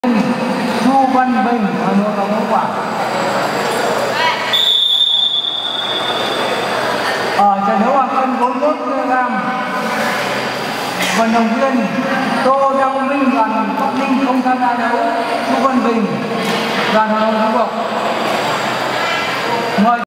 Giải các lứa tuổi trẻ 2019: Công bố Tô Đăng Minh B Ninh VS Chu Văn Bình H Nội
Video được trang fanpage Vật Việt Nam Vietnam Wrestling quay trực tiếp tại nhà thi đấu Thanh thiếu niên Tuyên Quang, tháng 6 năm 2019